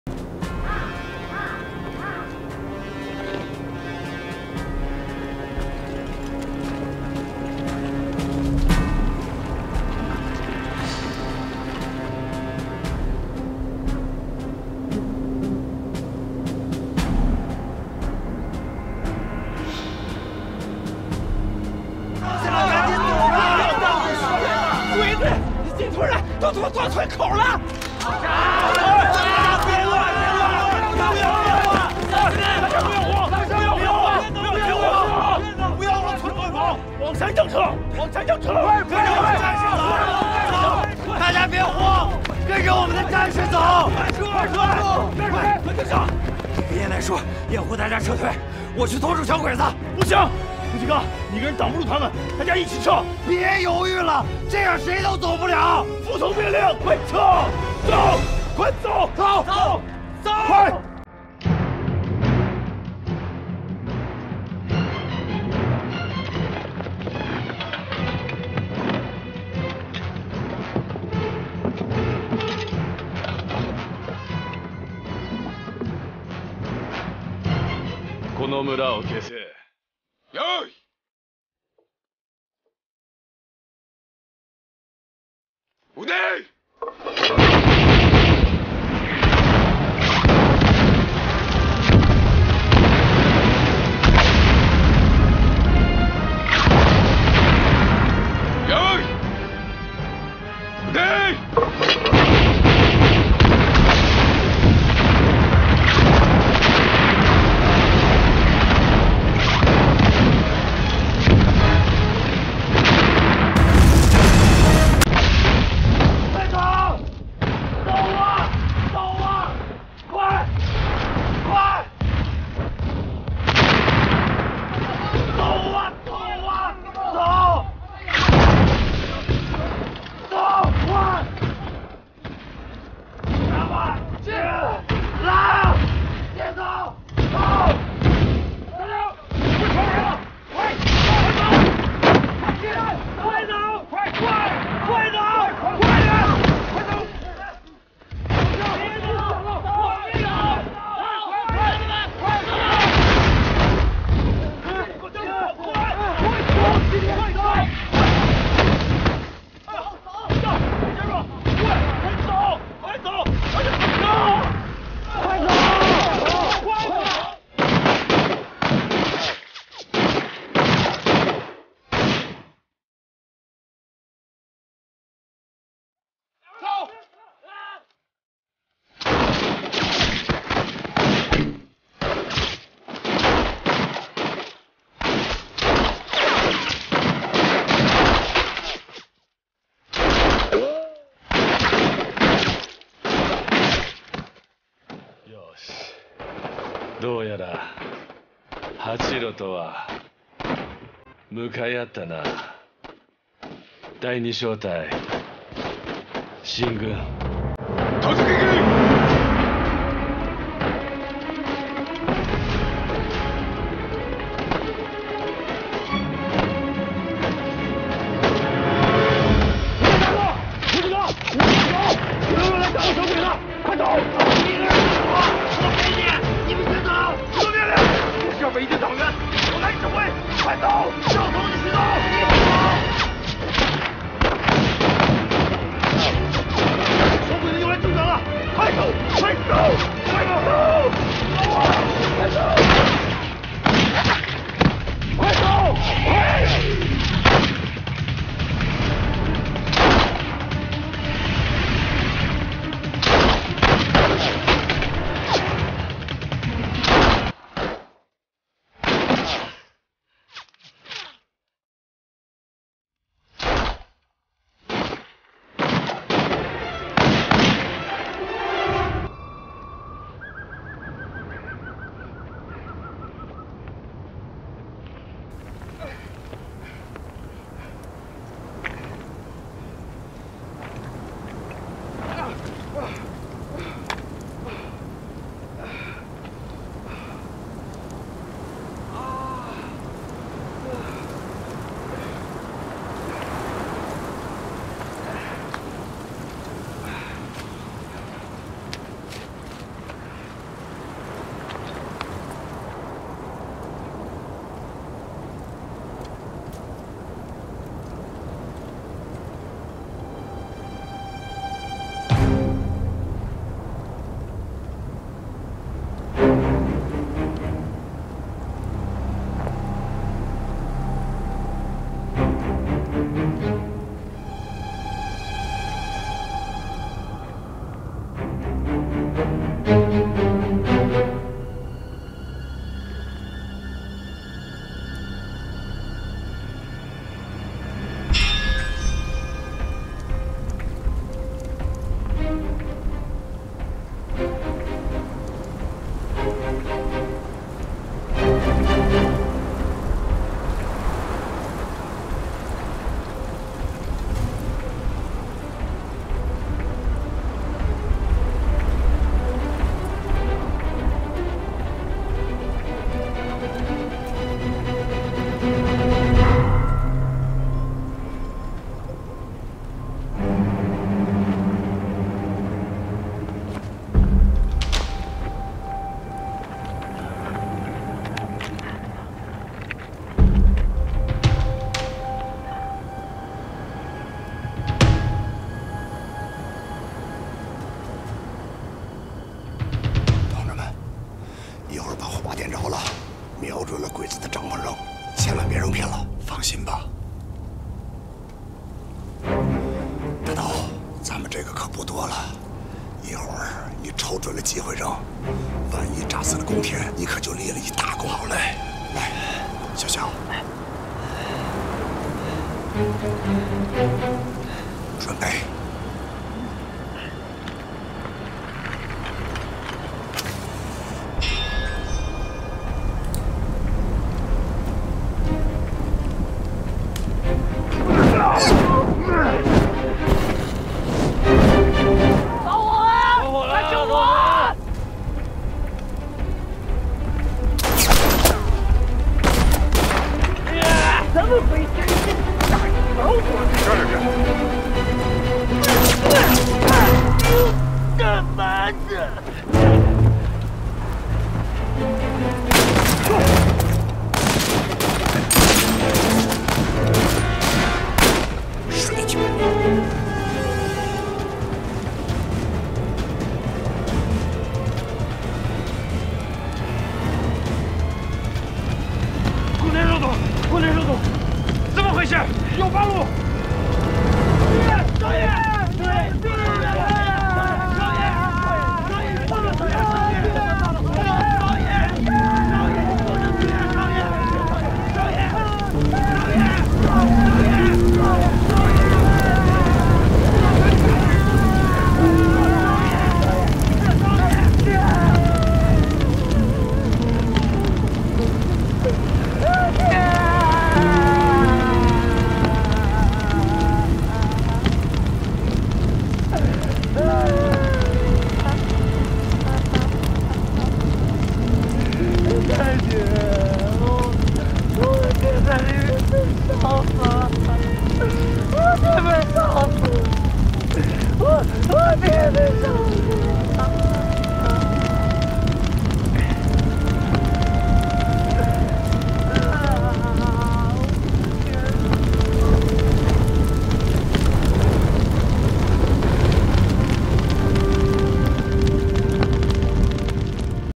日軍迫擊炮屠村，八路夜襲日軍營地報仇 ⚔️ 抗日
sub_confirmation=1

正版授权，侵权必究！

JDFJZZ